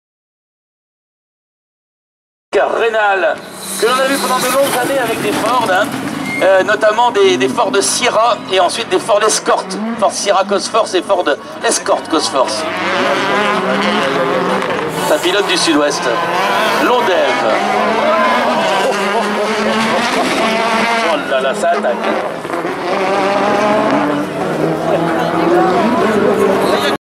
Limit Crash Rallye du Touquet 2016
Spéciale 5 du Front de mer, crash évité de justesse entre la FORD FIESTA R5 et la DS3 R5